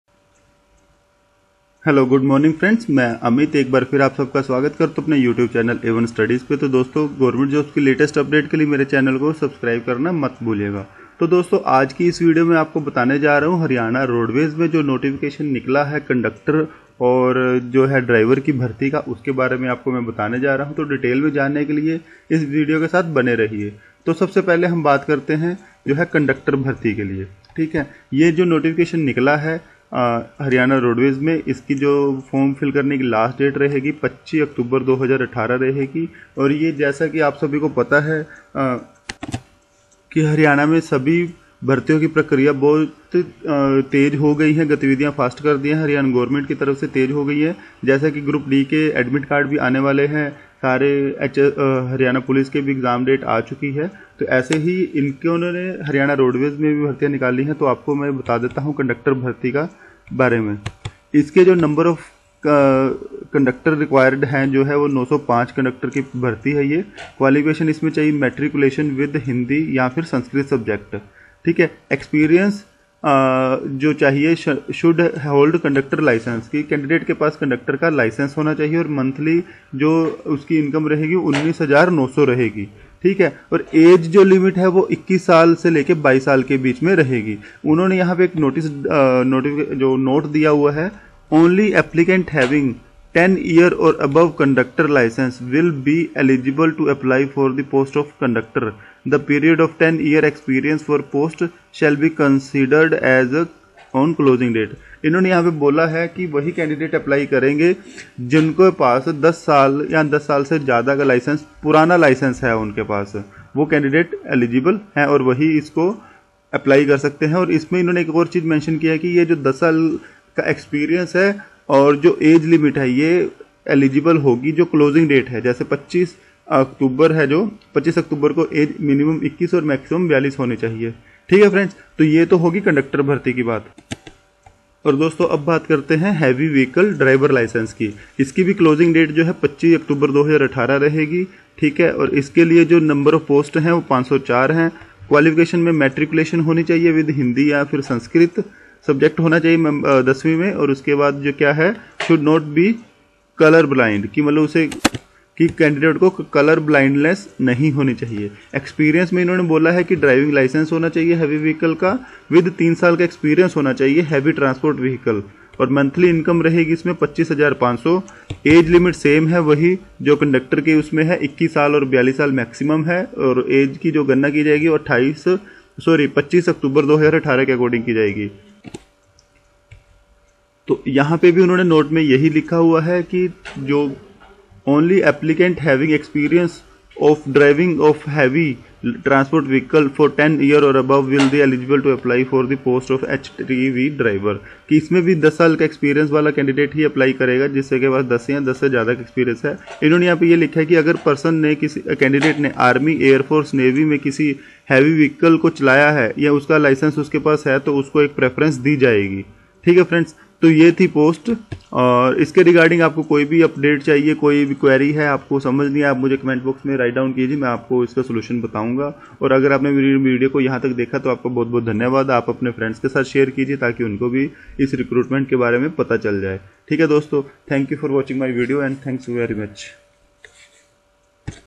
Haryana Roadways Notification Out // Last date 25 Oct 2018
Haryana Roadways Recruitment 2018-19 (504 Driver and 905 Conductor Vacancy)